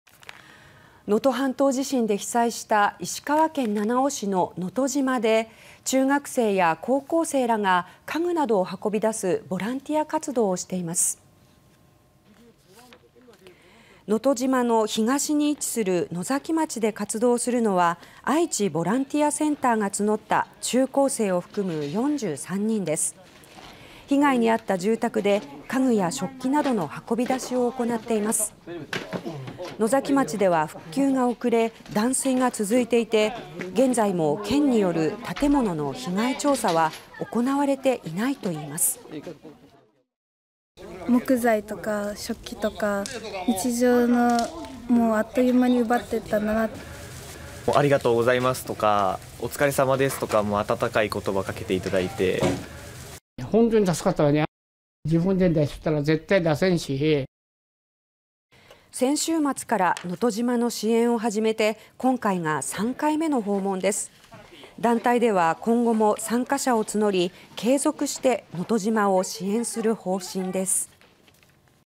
地震で被災の石川・能登島 中高生らがボランティア活動(2024年2月18日)
能登半島地震で被災した、石川県七尾市の能登島で中学生や高校生らが家具などを運び出すボランティア活動をしています。

　能登島の東に位置する野崎町で活動するのは、愛知ボランティアセンターが募った中高生を含む43人です。

　被害に遭った住宅で、家具や食器などの運び出しを行っています。

　野崎町では復旧が遅れ、断水が続いていて現在も県による建物の被害調査は行われていないといいます。

ボランティアに参加した高校生
「木材とか食器とか、日常をあっという間に奪っていった」
「ありがとうございますと。温かい言葉をかけていただいて」

被災した男性
「本当に助かった。自分では（物を）出せないし」

　先週末から能登島の支援を始めて、今回が3回目の訪問です。

　団体では今後も参加者を募り、継続して能登島を支援する方針です。